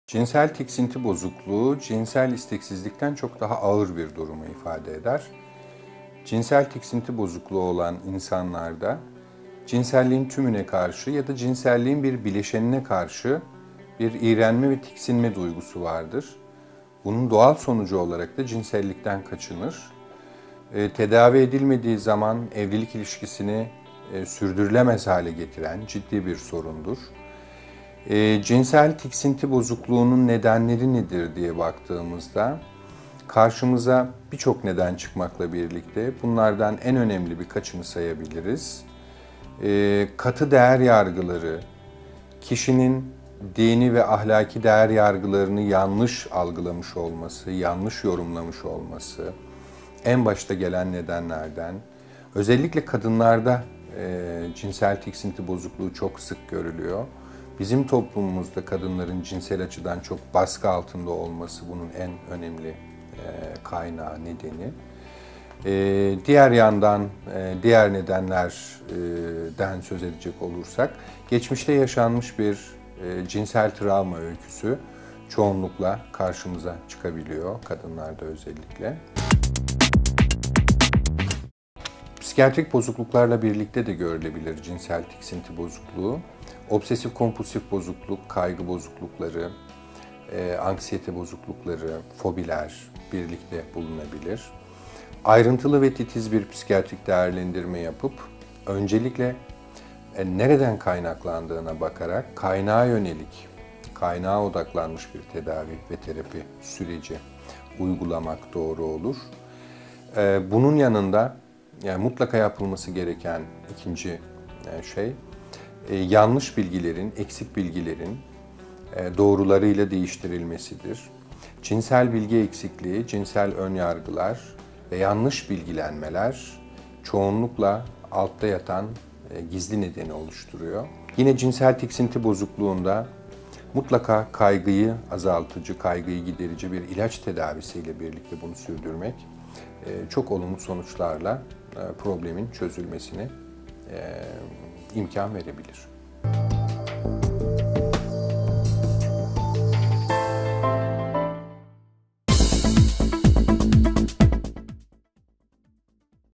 Cinsel tiksinti nedir? Cinselliği nasıl etkiler?
Cinsellik ve psikoloji hakkinda merak ettikleriniz... Ne tür psikolojik nedenler cinselligi olumsuz etkiler? Hangi durumlarda cinsel hayat olumsuz etkilenir? Evlilikte yasanan sorunlar cinselligi olumsuz etkiler mi? Antidepresan kullanimi cinselligi olumsuz etkileyebilir mi?